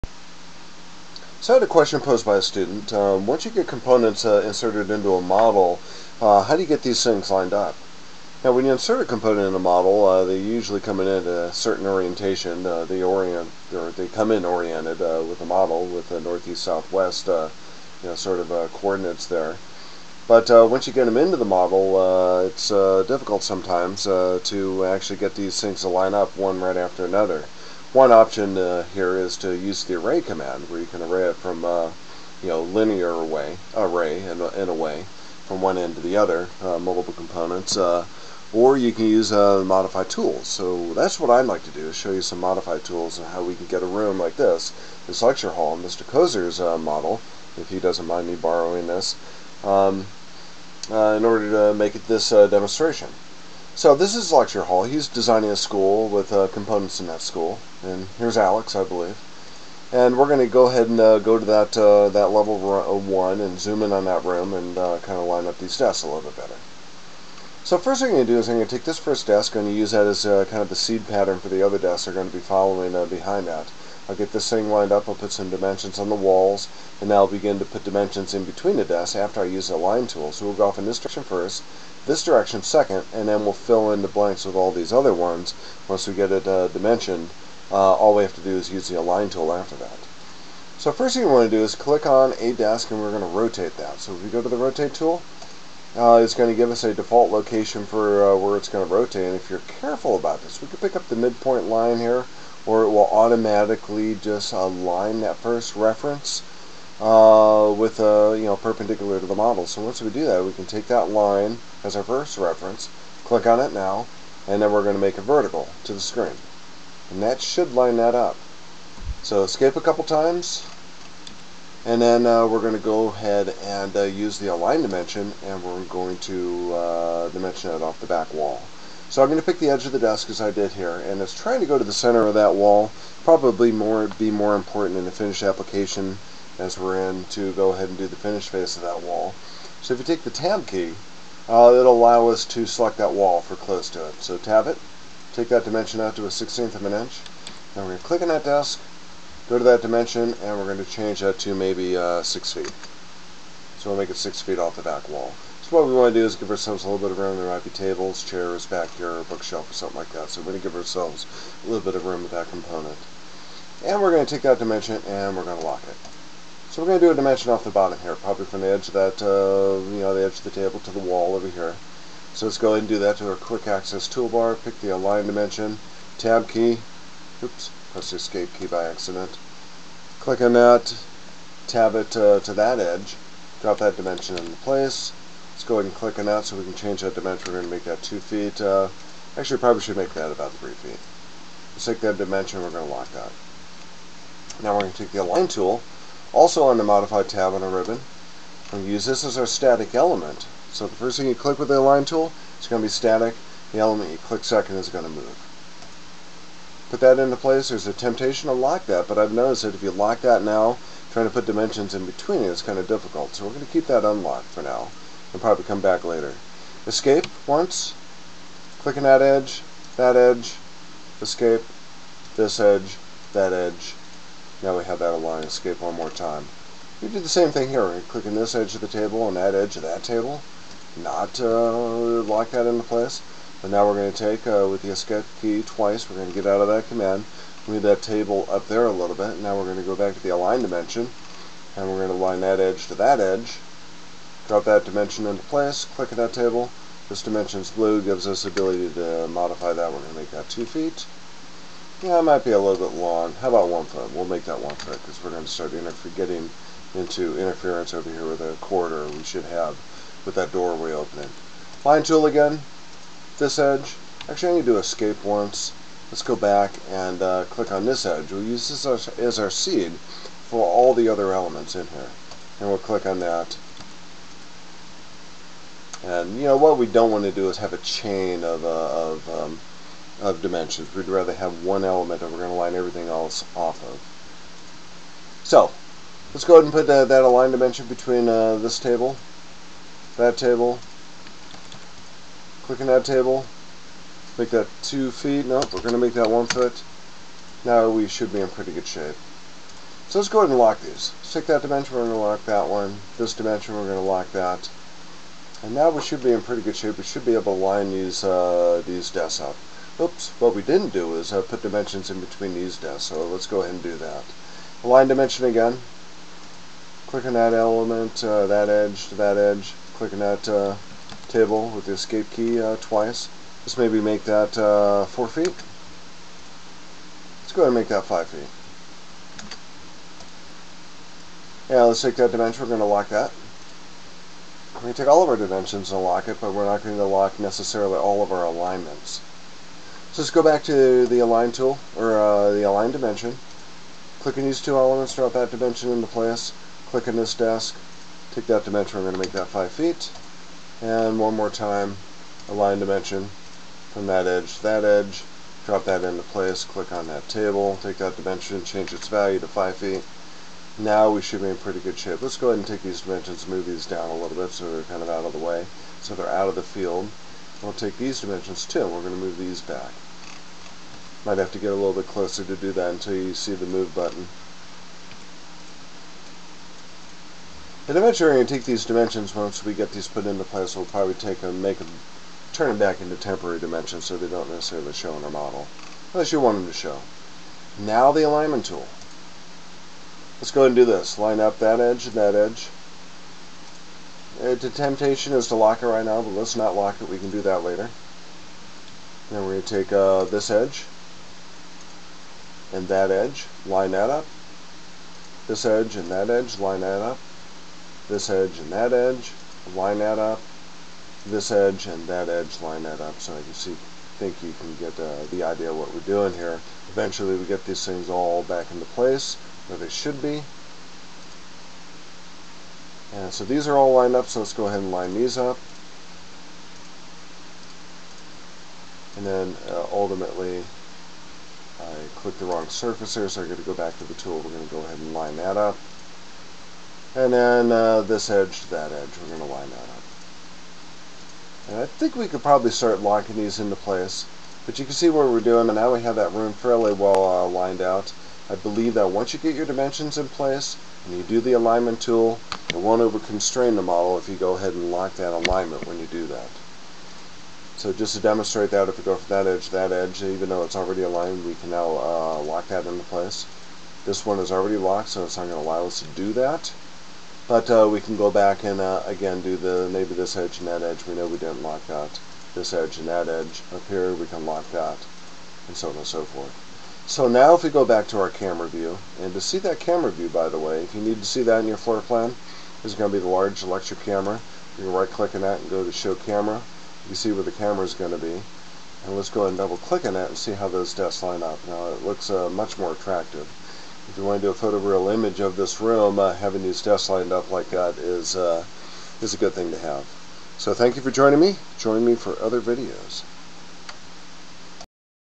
Revit, Aligning and Locking Inserted Components and Dimensions
Revit Architecture.  This video describes how to line up various inserted components in a model so that they have a coplanar surfaces and a specific distance from each other.  Tools used include the Align Tool, Dimensions and the Lock.  Tutorial for IET 161, Architectural CAD class in Central Washington University's Construction Management Program.